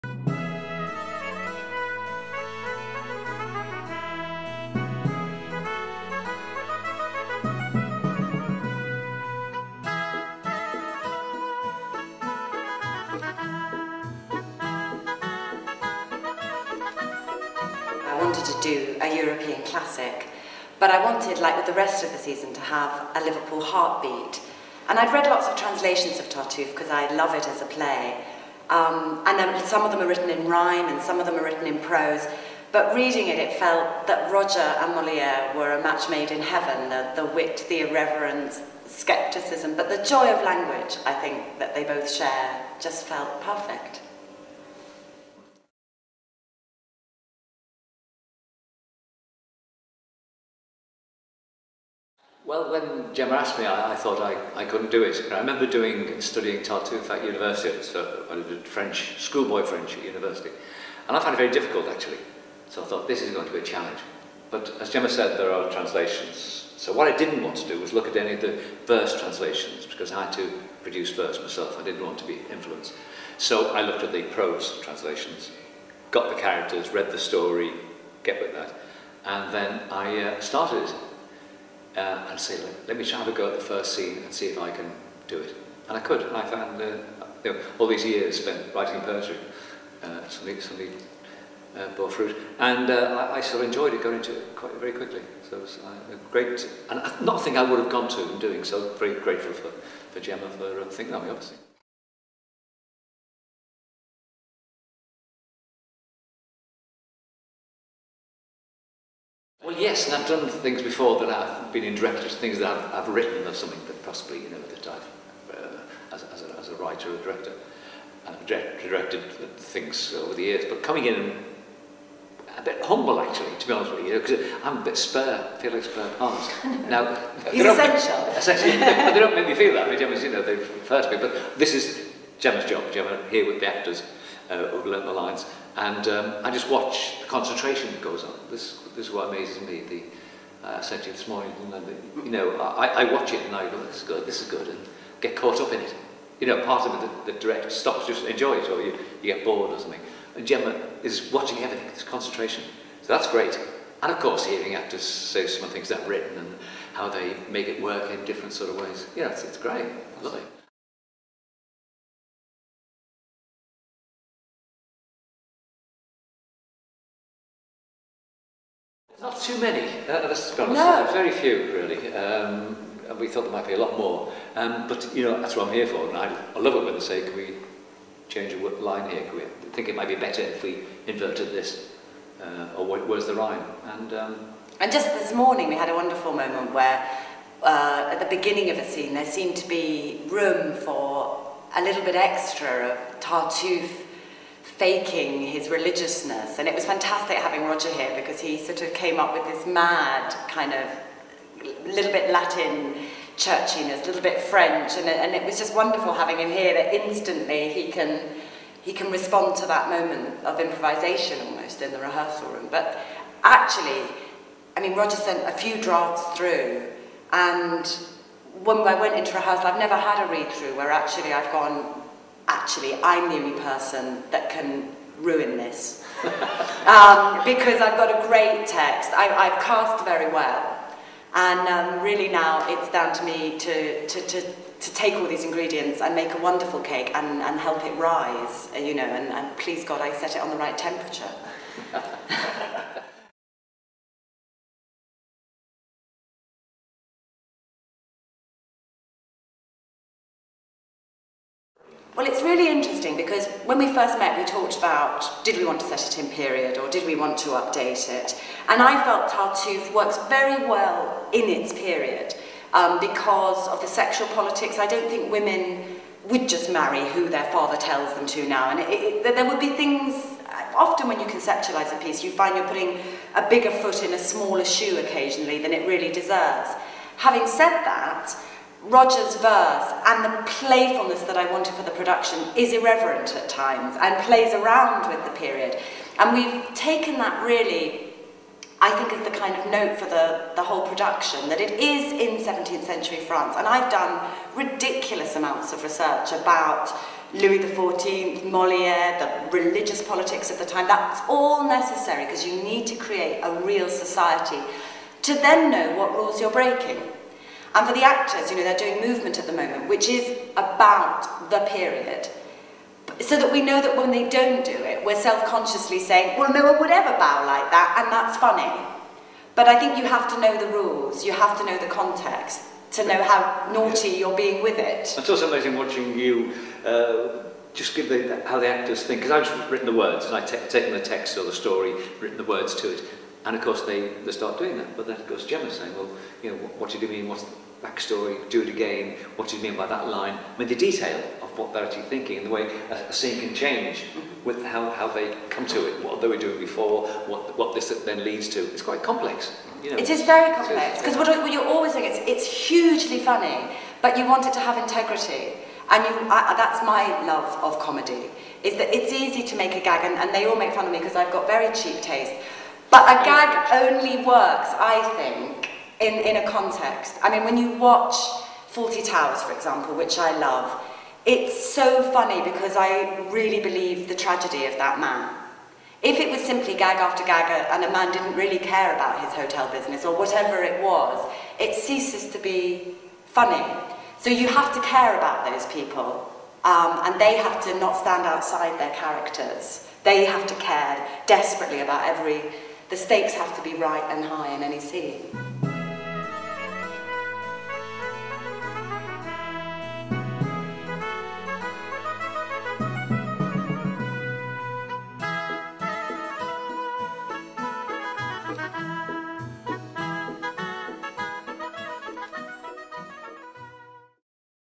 Tartuffe - Gemma & Roger
Director Gemma Bodinetz & Poet Roger McGough discuss the production.

Originally banned by Louis XIV, Tartuffe is Moliere's famous comedy re-told in a sparkling and witty version by 'Liverpool's Poet Laureate', Roger McGough.

Tartuffe is a beacon of piety and in the home of wealthy merchant Orgon he has his feet firmly under the table. But all is not as it seems and as Orgon becomes more enraptured with is new companion the whole city is chattering. Is he a friend, a fraud, a miracle or a hypocrite?

The family smell a rat and amidst the frills and frivolity of Seventeenth Century society they hatch a cunning plan to outwit the wily deceiver before he brings their house crashing down.